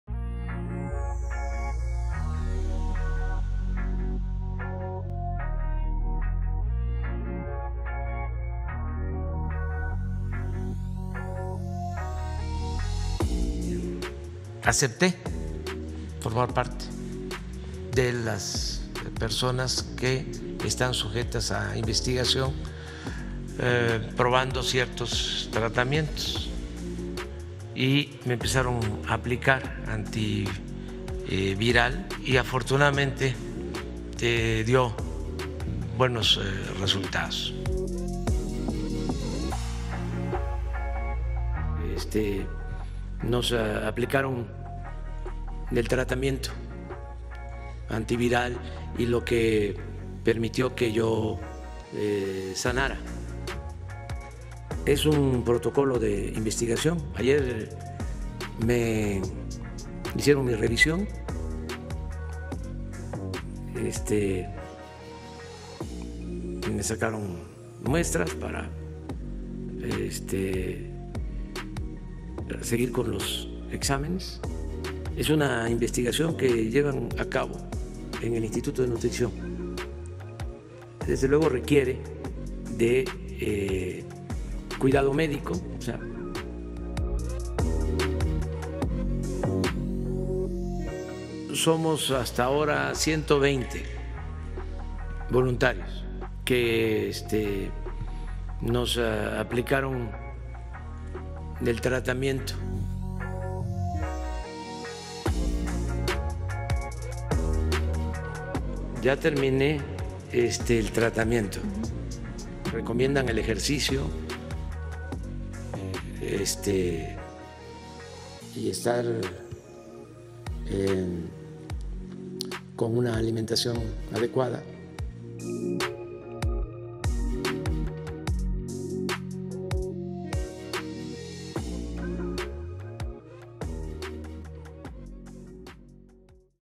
Así es el tratamiento experimental que recibió AMLO por COVID-19
El presidente Andrés Manuel López Obrador dio a conocer que el protocolo de investigación para el tratamiento de coronavirus o COVID-19 que él recibió, también participan 120 voluntarios e inclusive pudiera estar abierto para quien lo solicite. ¿de qué va?